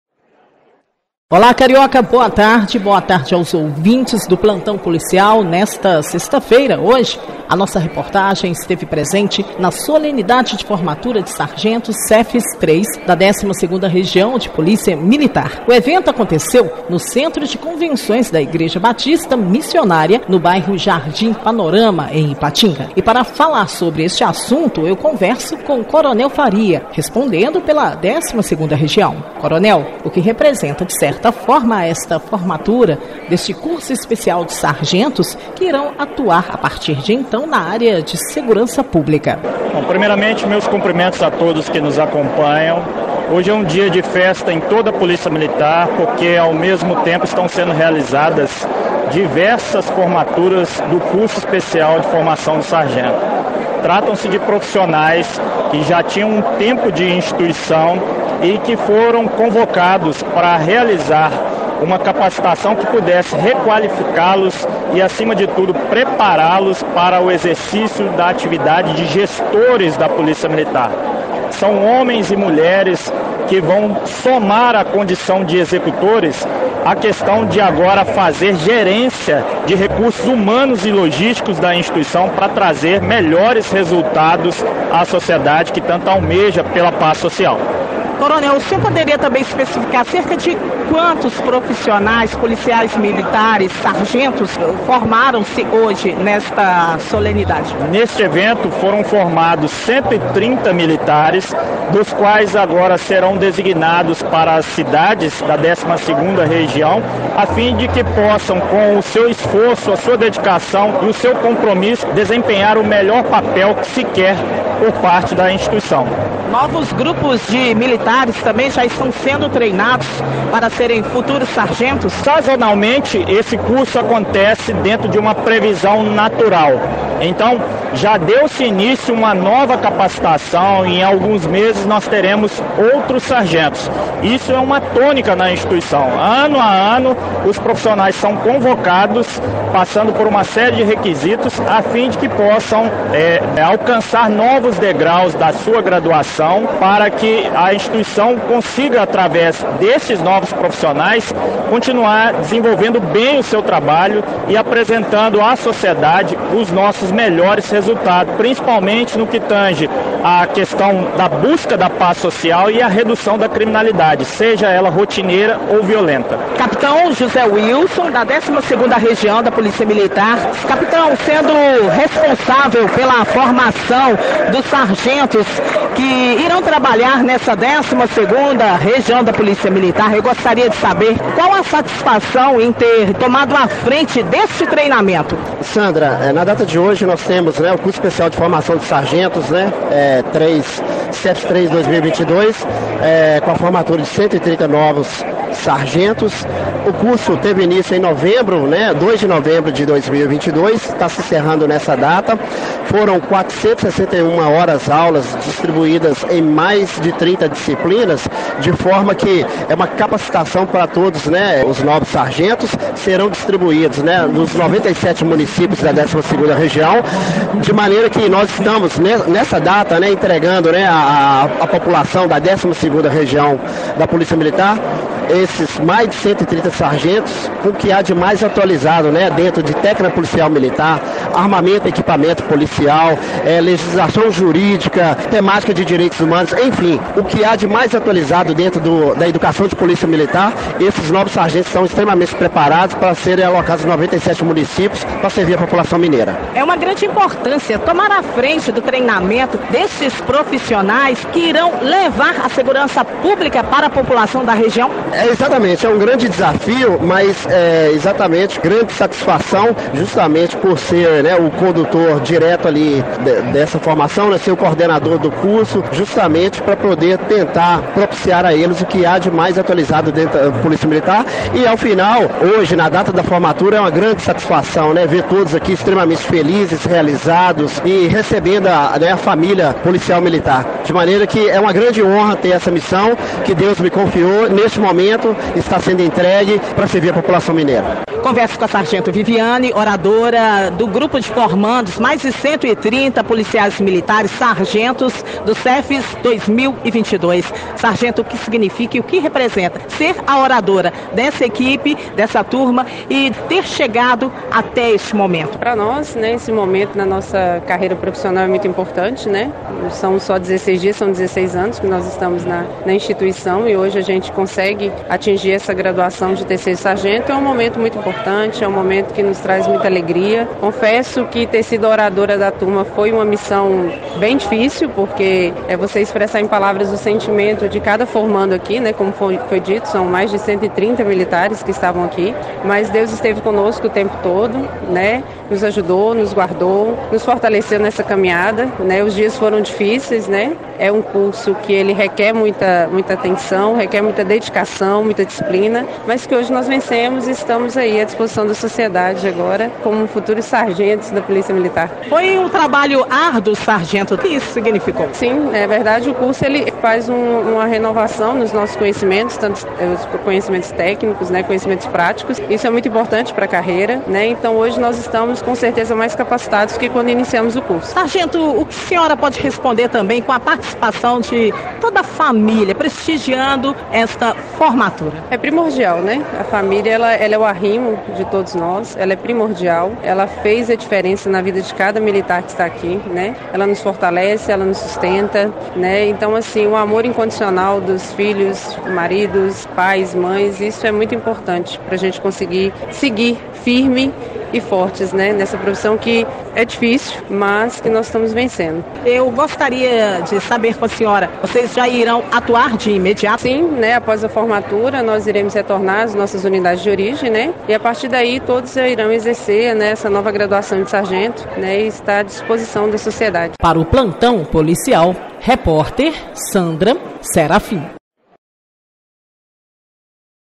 12ª RPM forma 130 novos sargentos
IPATINGA - A 12ª Região de Polícia Militar promoveu nesta sexta-feira, (24/03), a formatura do Curso Especial de Formação de Sargentos (CEFS-III), que teve a participação de 130 formandos. Esses militares já fazem parte das sete unidades operacionais da RPM, que é responsável por 97 municípios, e passaram cinco meses nesse treinamento, que mesclou todo o arcabouço teórico da PM, com as disciplinas práticas.

A solenidade foi presidida pelo Coronel Rodrigo de Faria Mendes, comandante da 8ª RPM, que está respondendo pelo comando da 12ª RPM, ele estava acompanhado de autoridades militares e civis.

O Curso Especial de Formação de Sargentos (CEFS) foi criado com o intuito de qualificar o público interno, para que os militares exerçam as atribuições do cargo de sargento.

O Coronel Faria explica a dinâmica do curso, e como esses novos sargentos devem se portar diante dos desafios na carreira. “Os nossos sargentos foram qualificados, capacitados e treinados para desenvolver, principalmente, a função de gestores da instituição.

A sociedade agora, recebe novamente esses profissionais, mais capacitados para servir, bem, o povo mineiro. Esses militares já estavam na instituição, e depois que alcançaram um determinado tempo de serviço, foram inseridos no curso.

Eles deixam de fazer o serviço de executores na ponta da linha, e agora se aproveitam de todo o conhecimento adquirido para fazer o papel de gestores do recurso humano, bem como dos recursos logísticos, tudo para apresentar os melhores resultados para sociedade de bem, que quer viver em cidades protegidas, principalmente, pelo aparelhamento policial”.

O momento mais aguardado por todos, é a aposição das divisas, pelas madrinhas e padrinhos dos formandos. Após, os presentes ouviram o discurso da oradora da turma, a 3º Sargento Viviane Melo.

O paraninfo dos formandos, o prefeito de Santana do Paraíso, Bruno Morato, não pode comparecer por questões de saúde, mas foi representado pelo vice-prefeito José Anicío de Almeida, que recebeu uma lembrança dos novos sargentos.

A entrega do presente foi realizada pelo Coronel Faria, Tenente Coronel Ademir, Chefe do Estado Maior da RPM, e um representante dos formandos. O primeiro colocado no curso, o 3º Sargento Rodrigo Menezes foi homenageado pelo desempenho.

Outro momento aguardado por todos foi compromisso regulamentar à bandeira, que prega a execução das atribuições do cargo de sargento na promoção da paz social, e dedicar-se inteiramente ao serviço policial militar, mesmo com o sacrifício da própria vida.

Para a 3º Sargento Viviane, que foi oradora da turma de formandos, esse é um momento importante, já que depois de 16 anos na instituição, essa progressão na carreira coroa um bom trabalho realizado, e capacita para mais uma fase na PM, dessa vez com uma nova graduação.

“O curso faz uma renovação dos nossos conhecimentos, tanto técnicos quanto práticos, isso é relevante. Estamos muito mais capacitados. Após a formatura vamos retornar para nossas unidades de origem, e a partir daí já exerceremos, ainda melhores, as nossas novas atribuições”.